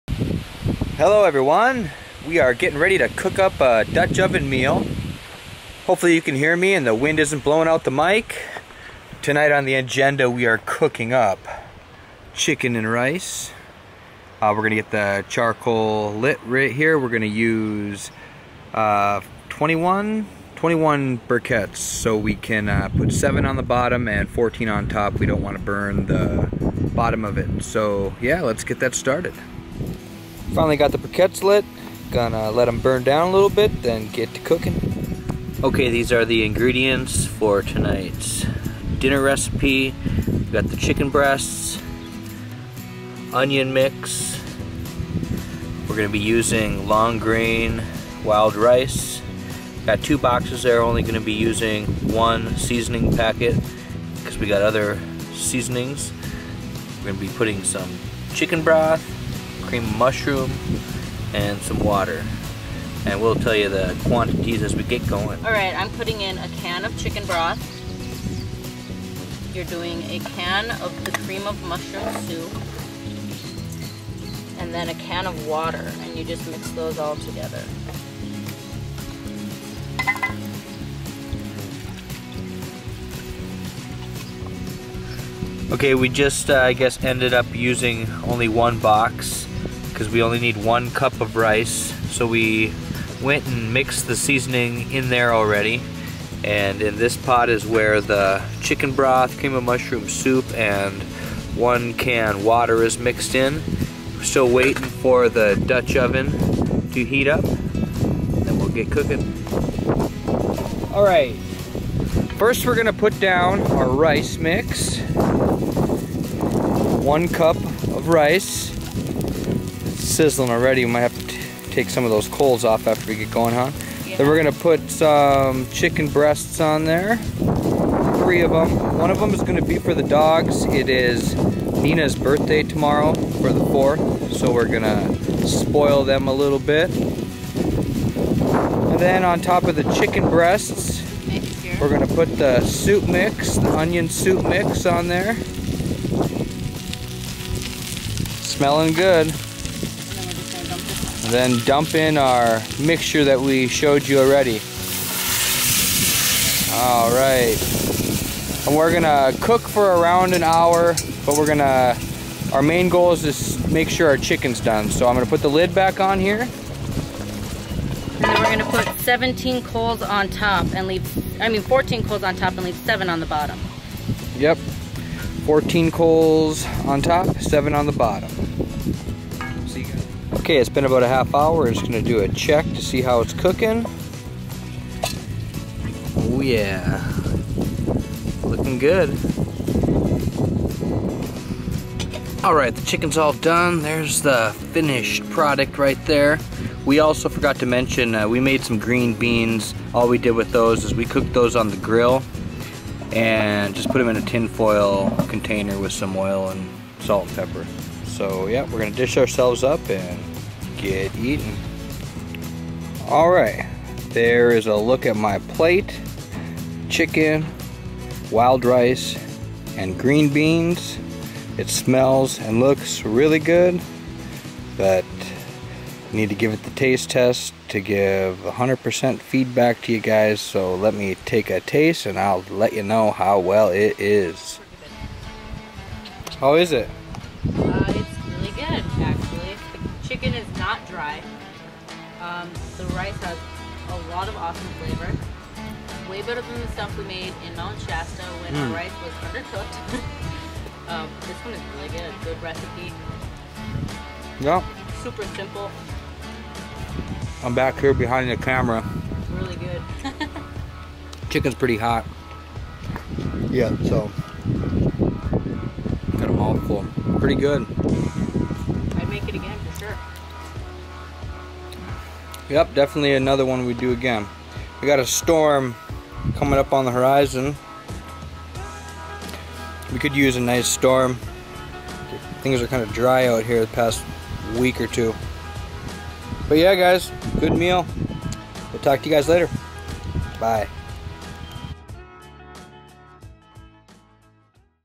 DUTCH OVEN RECIPE : BEST CHICKEN and RICE
Making chicken and rice in the dutch oven , the cast iron dutch oven is our favorite way to cook off grid . cooking off grid and the food taste so much better when cooked in the great outdoors . thank you all for watching